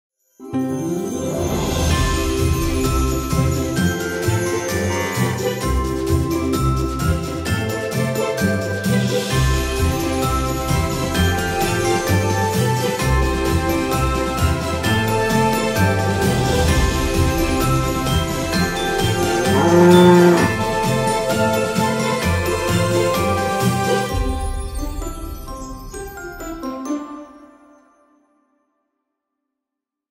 Milking the Cow Figurine | A Charming Christmas Scene | FG Square Villages
Add a touch of rural charm to your Christmas display with the Milking the Cow figurine from FG Square Villages. Explore our 2023 Christmas Collection for more heartwarming figurines. Find it at Menards or FGSquarecvillage.com.